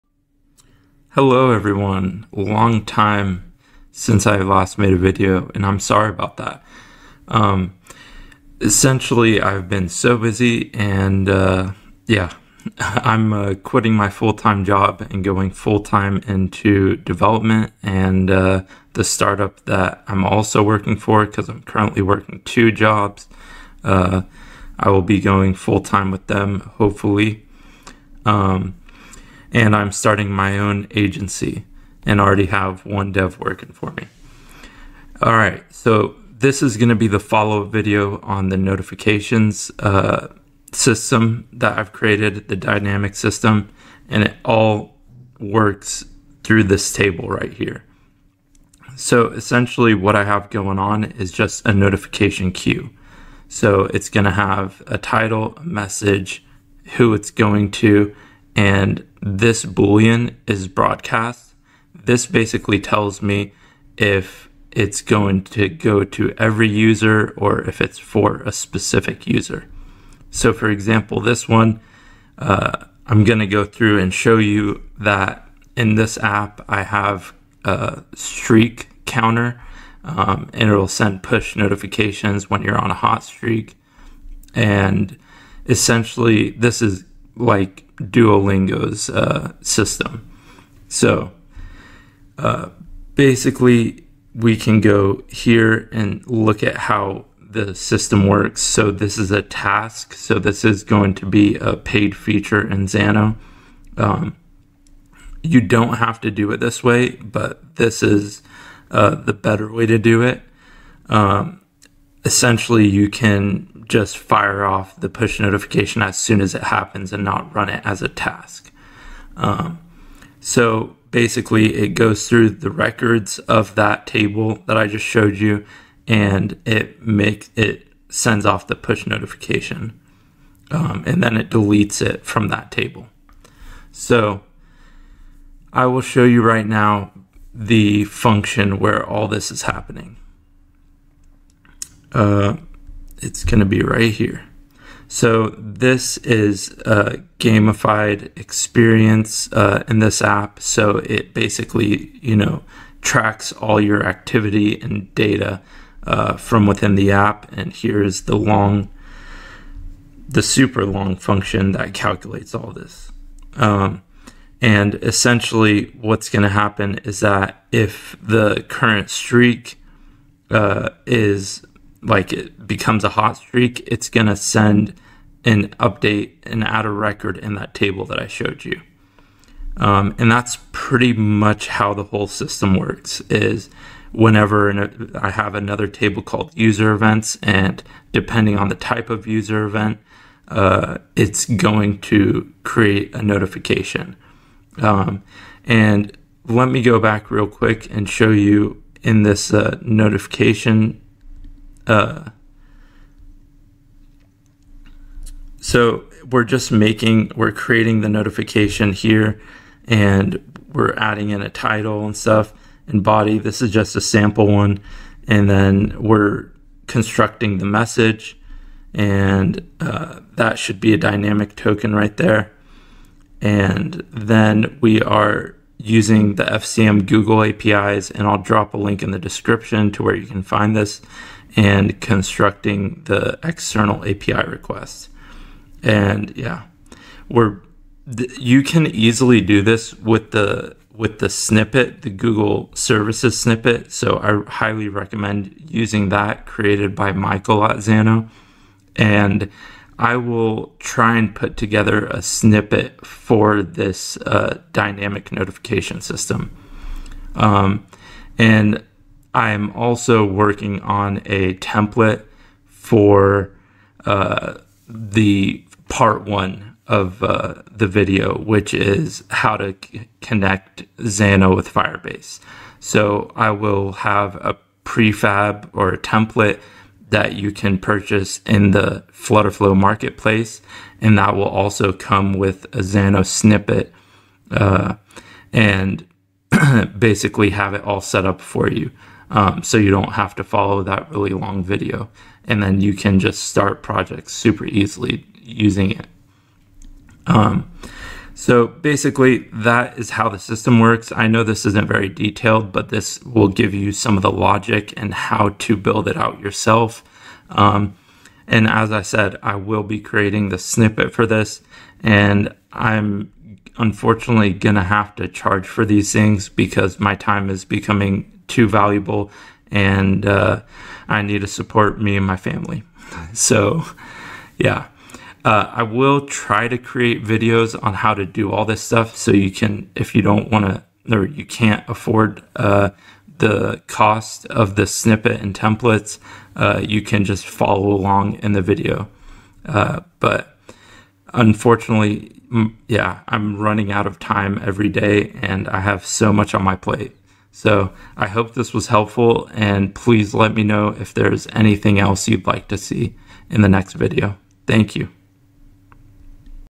[FlutterFlow] Xano Firebase Dynamic Push Notification System
Hey everyone! Sorry for not uploading in a while. I've been busy with client work, coaching, consulting, and my 2 jobs.

I hope this video gives you a good mental model on how to implement this and helps you get started on your push notification system in Xano.

I'll update y'all with the templates and snippets on Twitter/X so be sure to follow me there!

Let me know what else you'd like to see and I'll do my best to build it and put a video out for it.

Also, please let me know if this format is helpful or if a step-by-step live build is better.

- Daniel